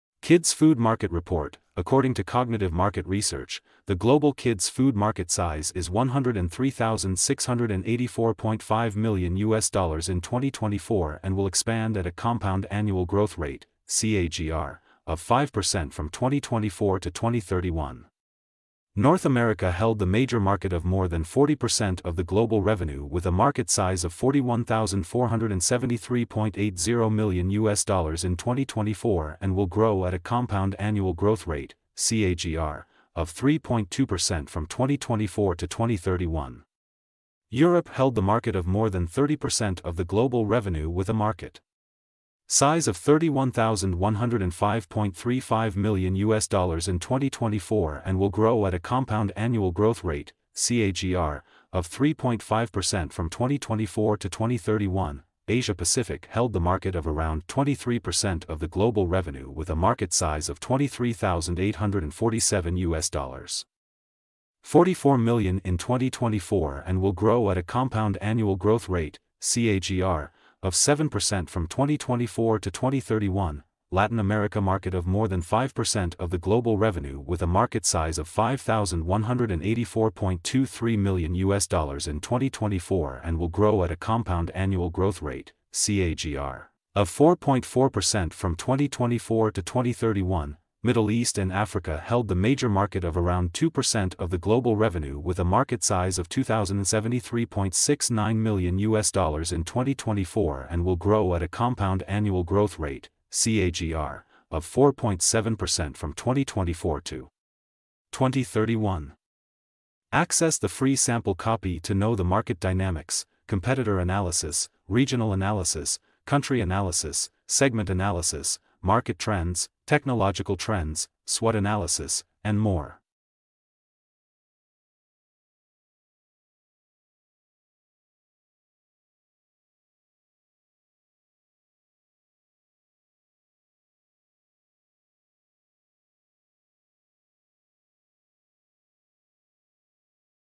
Kids Food Market Report 2024 | Forecast, Market Size & Growth
Report Insights: According to Cognitive Market Research, the global kids food market size is USD 103684.5 million in 2024 and will expand at a compound annual growth rate (CAGR) of 5.00% from 2024 to 2031.

North America held the major market of more than 40% of the global revenue with a market size of USD 41473.80 million in 2024 and will grow at a compound annual growth rate (CAGR) of 3.2% from 2024 to 2031.
Europe accounted for a share of over 30% of the global market size of USD 31105.35 million.
Asia Pacific held the market of around 23% of the global revenue with a market size of USD 23847.44 million in 2024 and will grow at a compound annual growth rate (CAGR) of 7.0% from 2024 to 2031.
Latin America market of more than 5% of the global revenue with a market size of USD 5184.23 million in 2024 and will grow at a compound annual growth rate (CAGR) of 4.4% from 2024 to 2031.
Middle East and Africa held the major market of around 2% of the global revenue with a market size of USD 2073.69 million in 2024 and will grow at a compound annual growth rate (CAGR) of 4.7% from 2024 to 2031.
The dairy products held the highest kids food market revenue share in 2024.